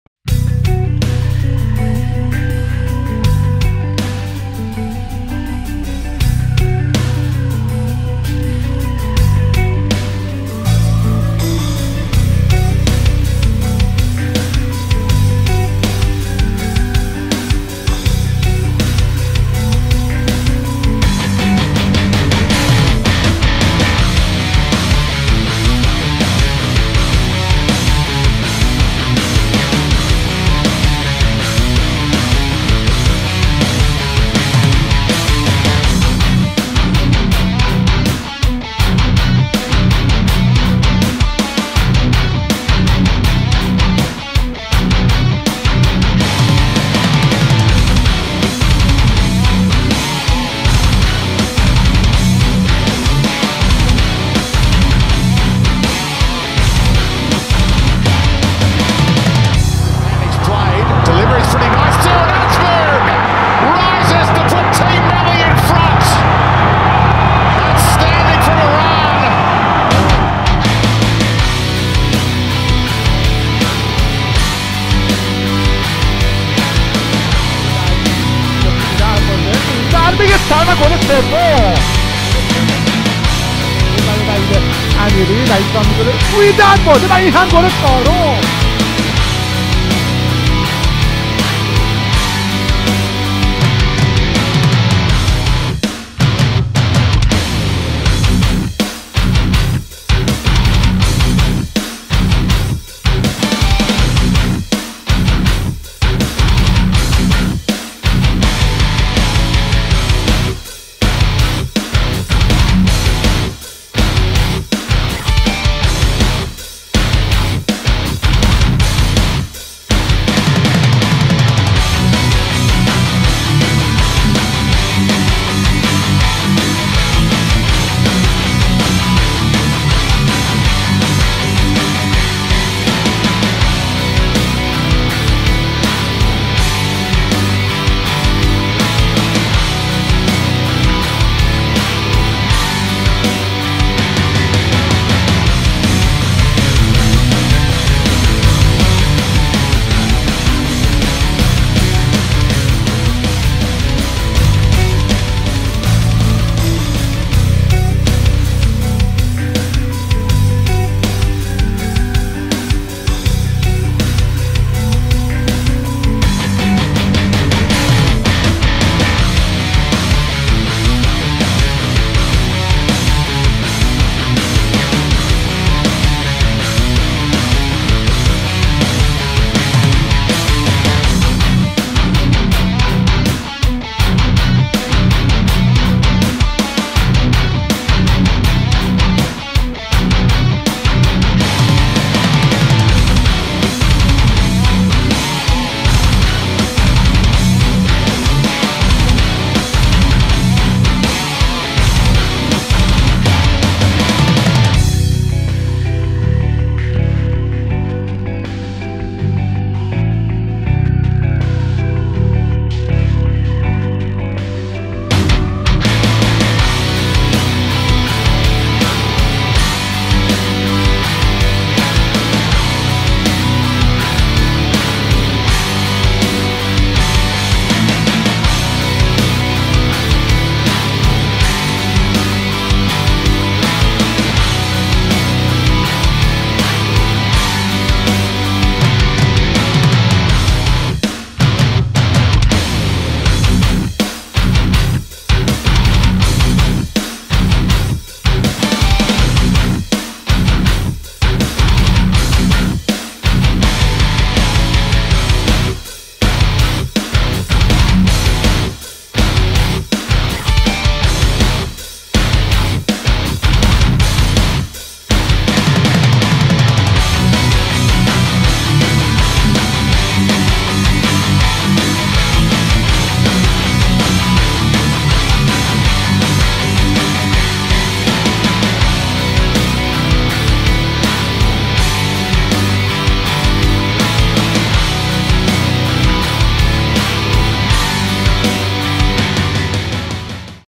Sardar Azmoun | All Goals for Iran (2014-2019)
All Sardar Azmoun goals for Team Melli Iran (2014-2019).

Sardar's 2 goals in the friendly vs. Qatar are NOT included (the match took place behind closed doors).